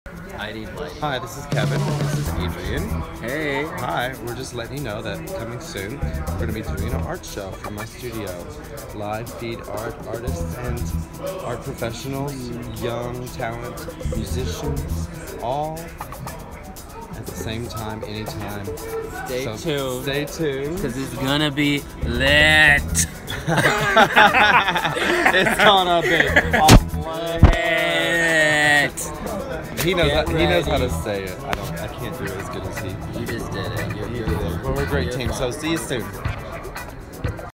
Art show coming soon
Kevin Baker and Adrian Smith talking the talk of big plans.